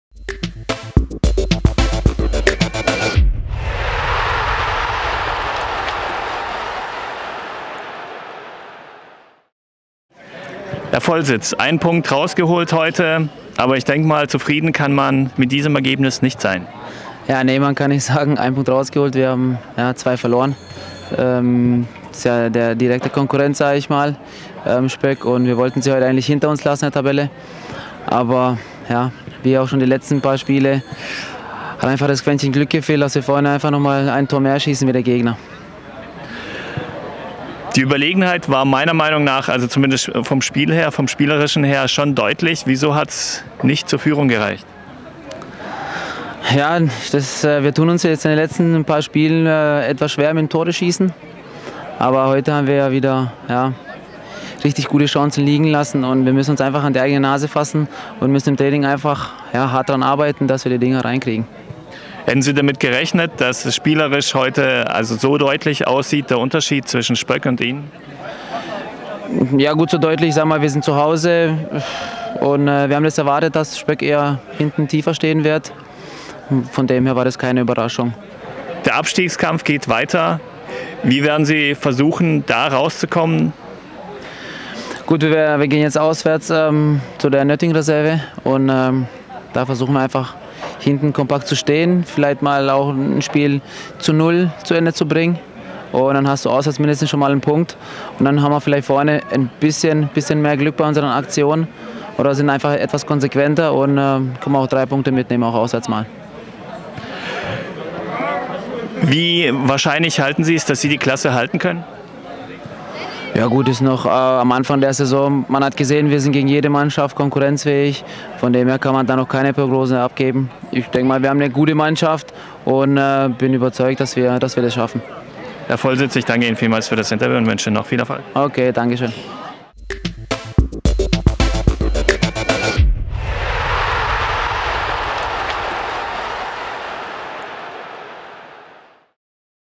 Norbert Volsitz - FSV Buckenberg - zum Spiel vs. FC Spöck im FuPa.tv-Interview am 16.10.2016
Norbert Volsitz, Chef-Trainer des FSV Buckenberg, im FuPa.tv-Interview nach der Begegnung FSV Buckenberg - FC Spöck am 9. Spieltag der Landesliga Mittelbaden 2016/17.